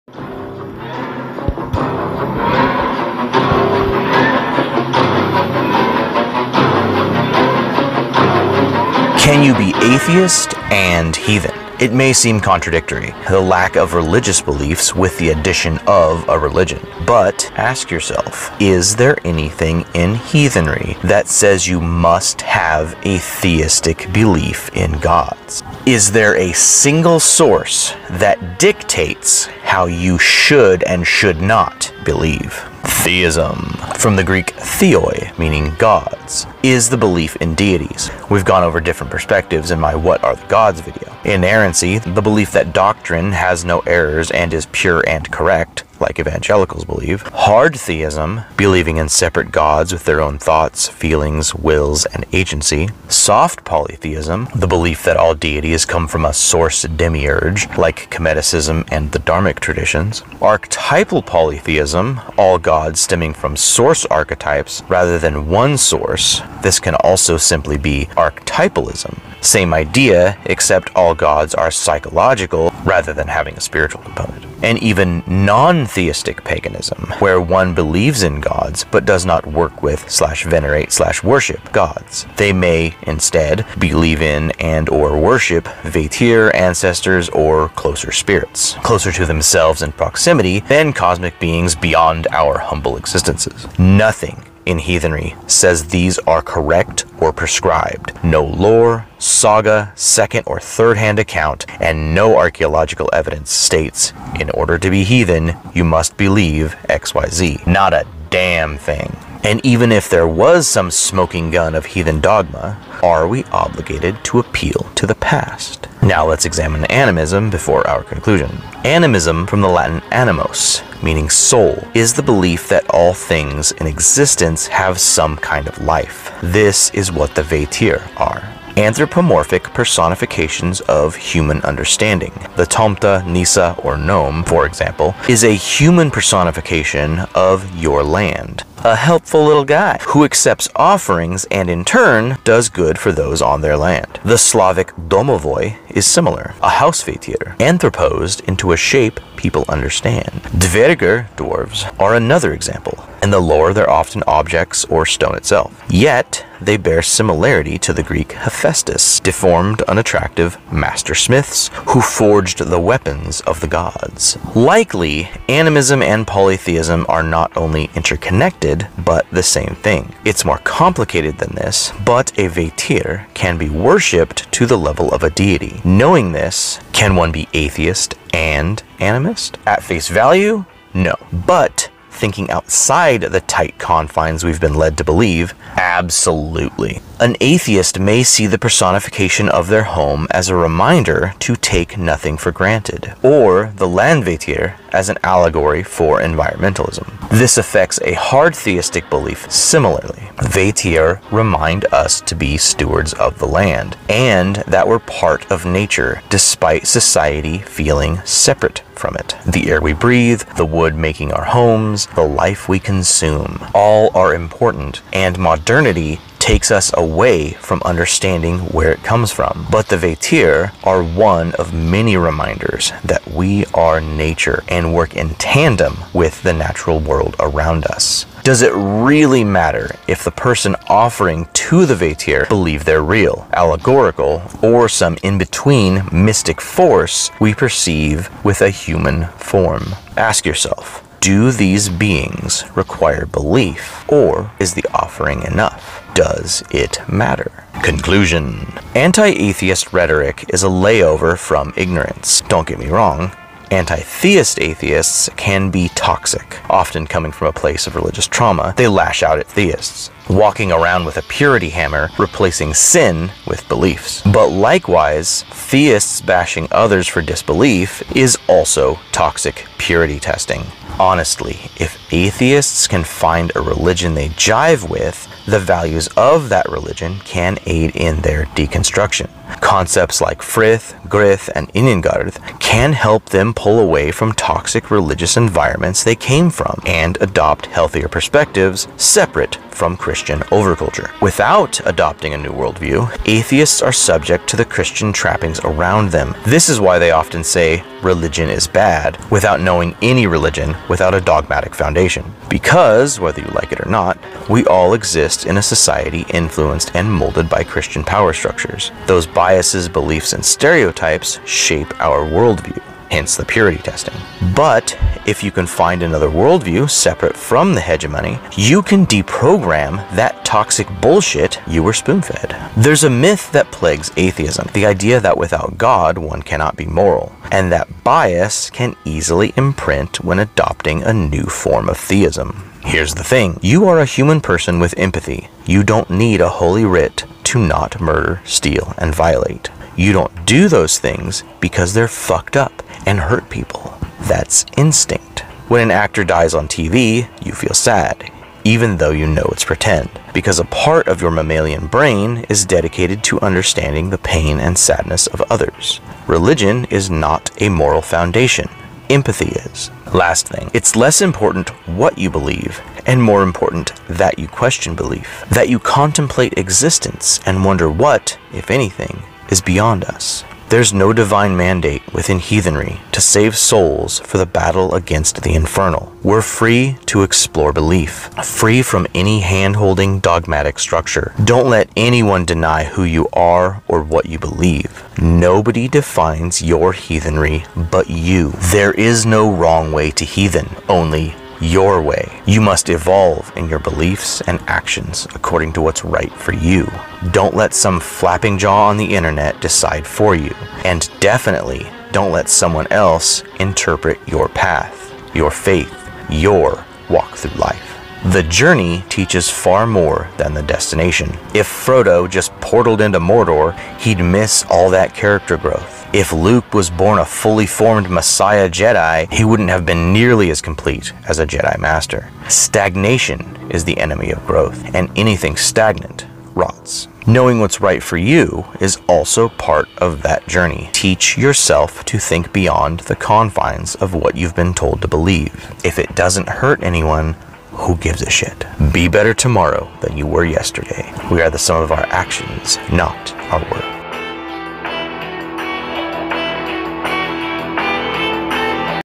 Can Atheists be Heathen?
Examining the beliefs of disbelief in relation to Heathenry